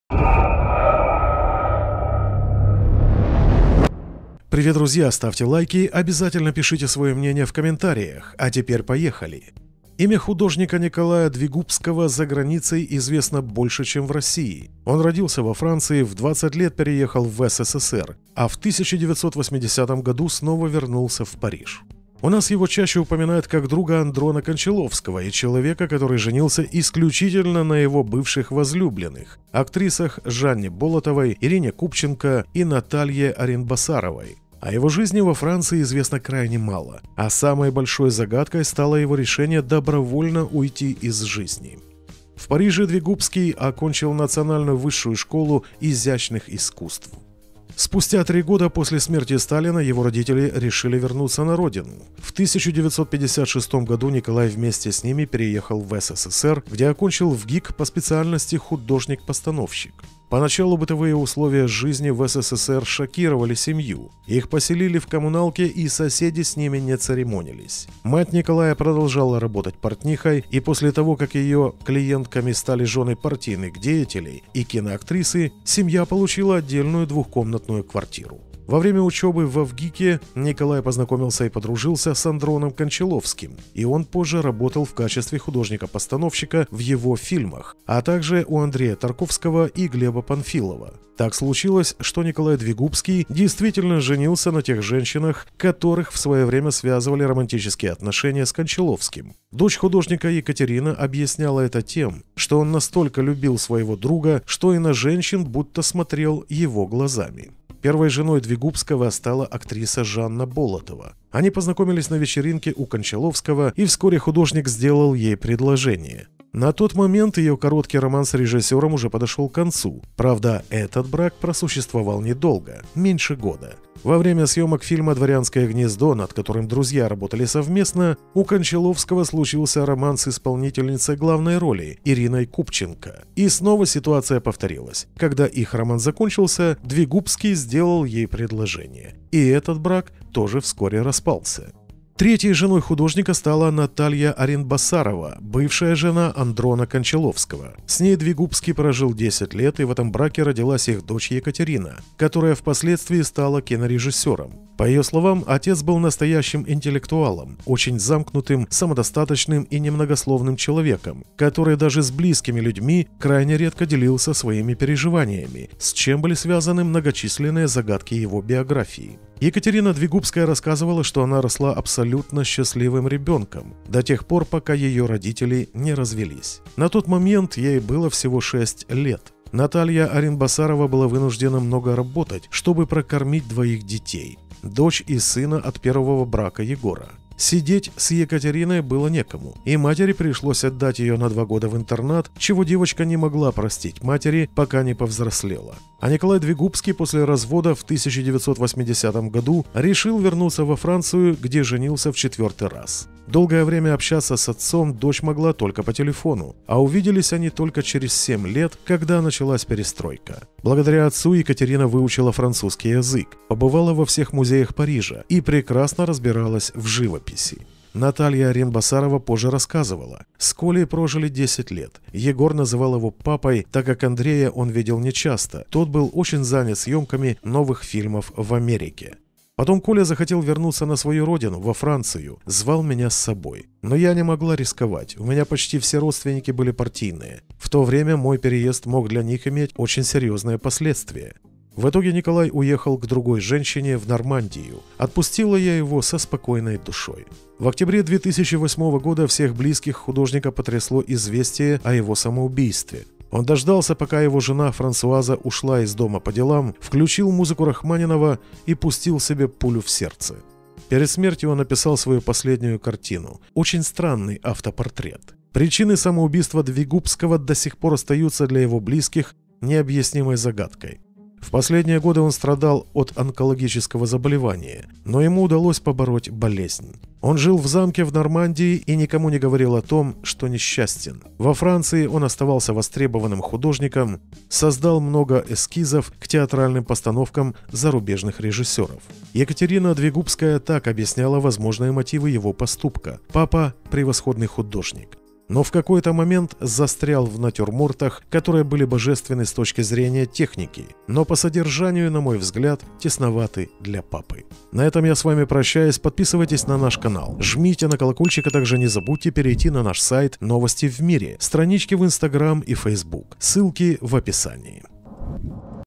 Перепутья Николая Двигубского: Загадка гибели бывшего мужа Ирины Купченко и Натальи Аринбасаровой
Имя художника Николая Двигубского за границей известно больше, чем в России – он родился во Франции, в 20 лет переехал в СССР, а 1980 г. снова вернулся в Париж. У нас его чаще упоминают как друга Андрона Кончаловского и человека, который женился исключительно на его бывших возлюбленных – актрисах Жанне Болотовой, Ирине Купченко и Наталье Аринбасаровой. О его жизни во Франции известно крайне мало, а самой большой загадкой стало его решение добровольно уйти из жизни…